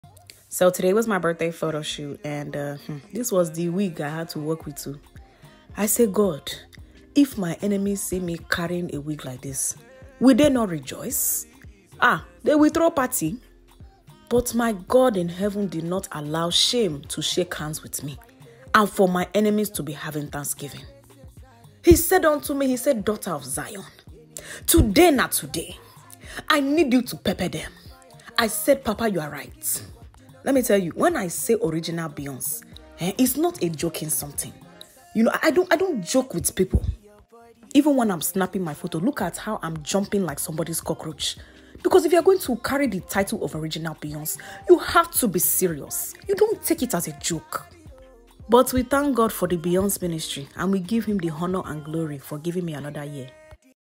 Birthday photo shoot (behind the scene)
A quick snippet of my birthday photo shoot and wig revival.